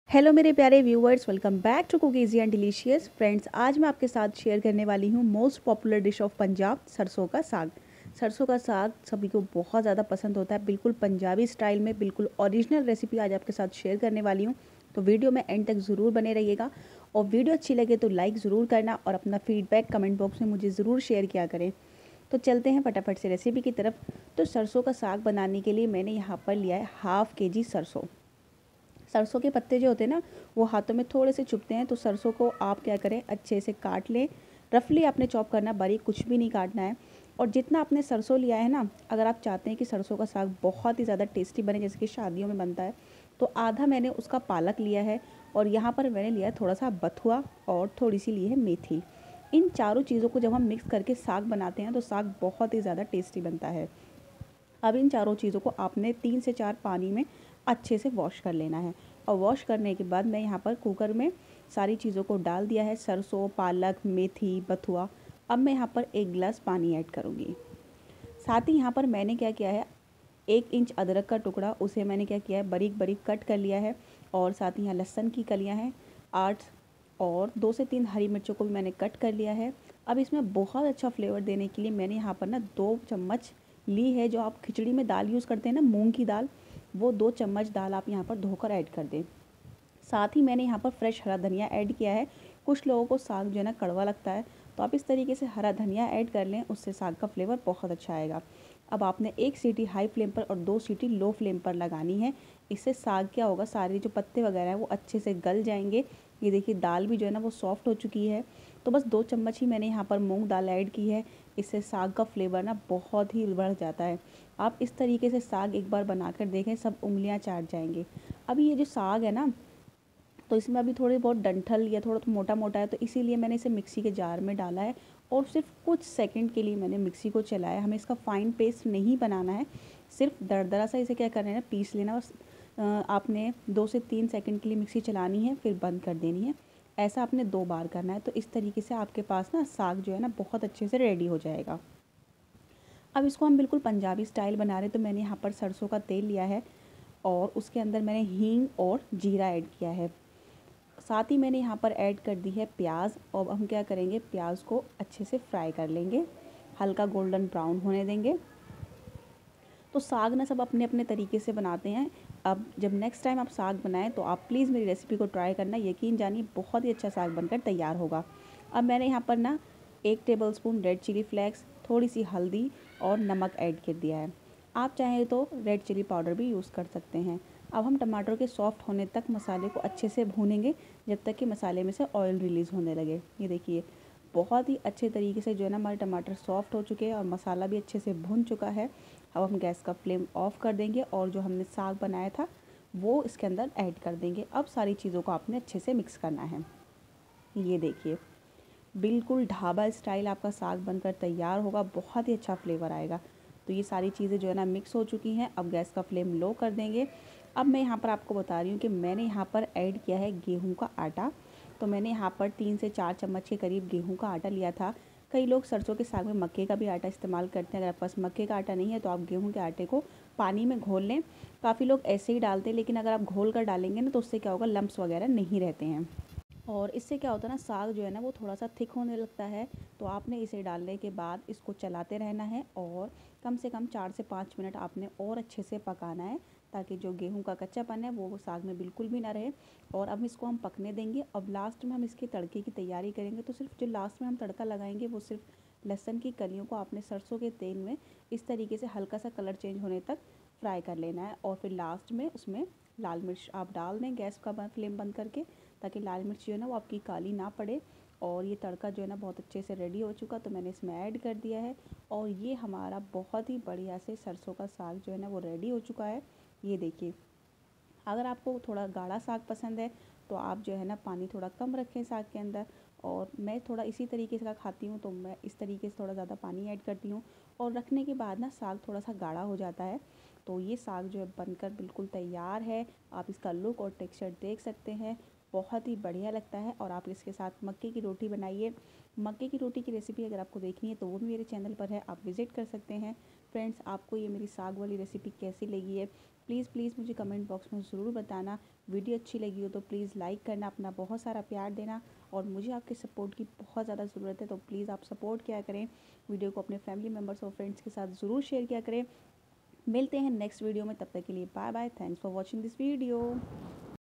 सरसों का साग पंजाब में कैसे बनाया जाता है ? Sarson Ka Saag । Punjabi style sarson ka saag।new recipe
साग कैसे बनाते हैं